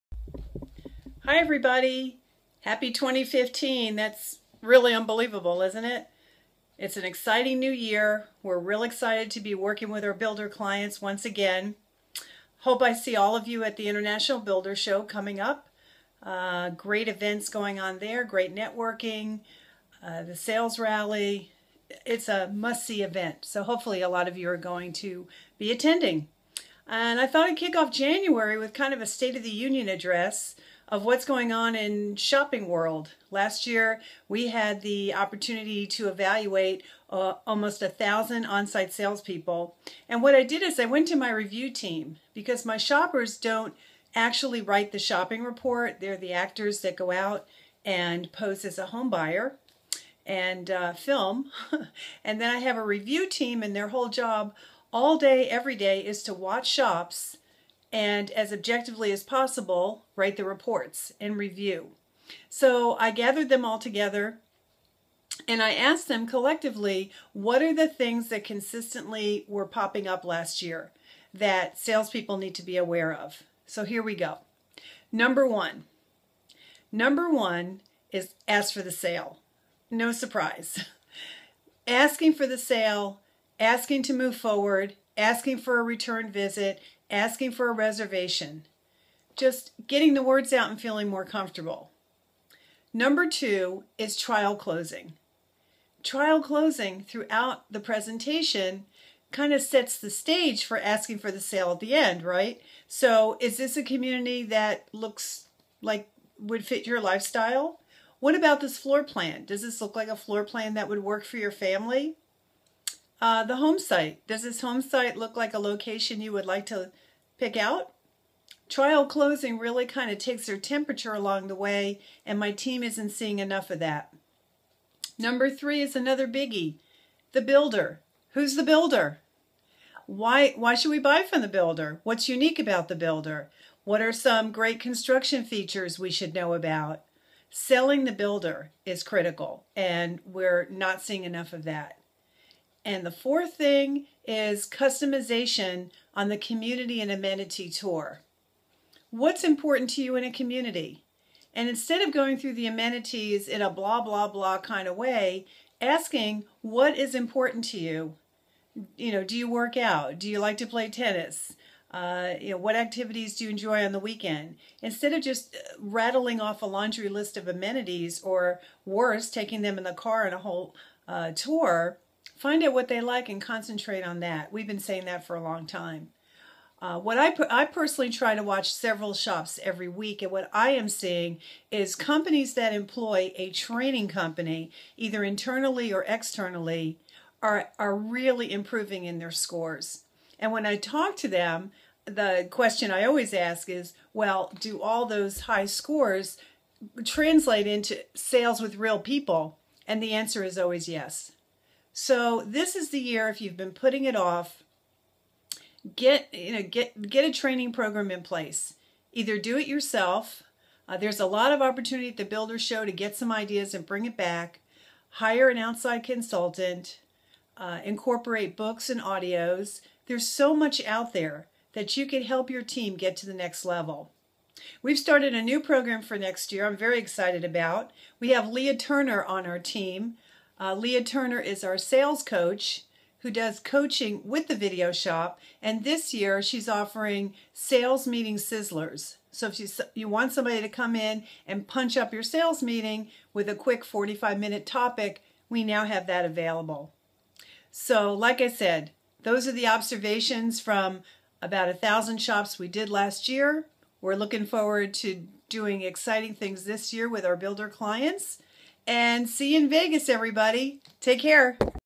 State of the Union Address in shopping world
The results from almost 1,000 video mystery shops of on site new home salespeople.